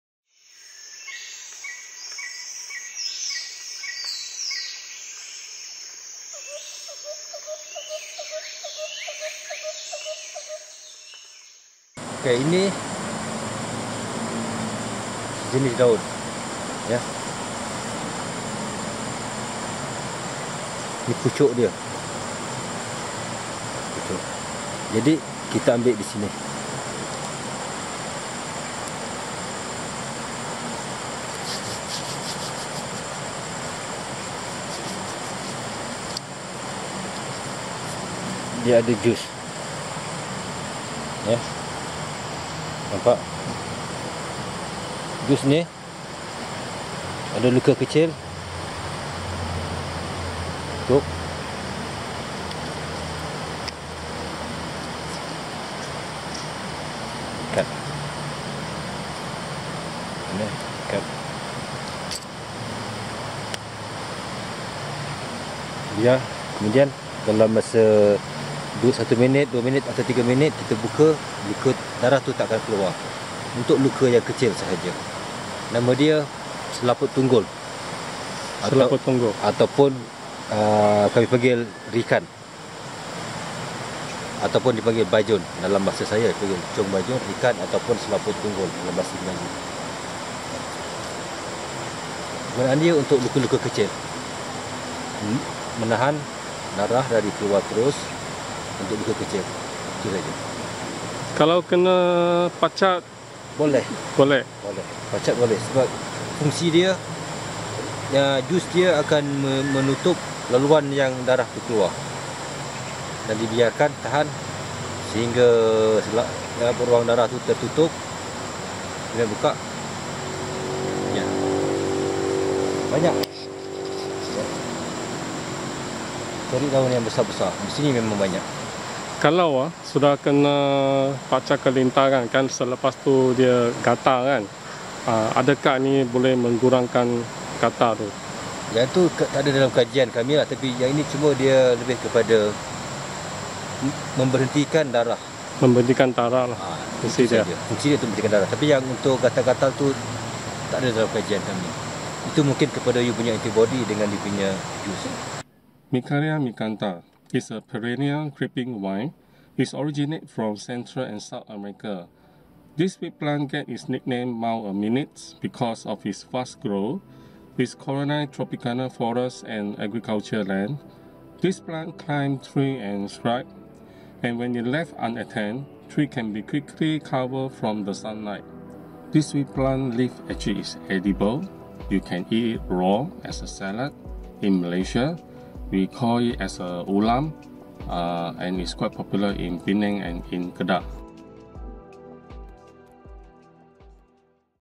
Jungle Survival Edible Food Part 4 - Mikania Micrantha (Selaput Tunggul)
Mikania micrantha Kunth (Asteraceae) is a perennial creeping vine. It originates from Central and South America. This weed plant gets its nickname "Mile-a-Minute" because of its fast growth. It colonise tropical forest and agricultural land. The plant climb trees and shrubs, and When left unattended, trees can be quickly covered from the sunlight.

The plant climbs by twining spirally around a support, which in the field is most often the main stem or a branch of a tree plant. The climber plant scales along the entire height of a the plant supporting it until the climbing plant reaches the top of the support plant. The climbing plant then grows profusely, producing leaves and flowers, thereby "suffocating" the support plant. The plant also climbs onto non-living support, whether it is a pole, dead trees or stumps. In Malay language, stump is equivalent to "tunggul". Hence, the plant is locally known as "selaput tunggul", which means "stump covering".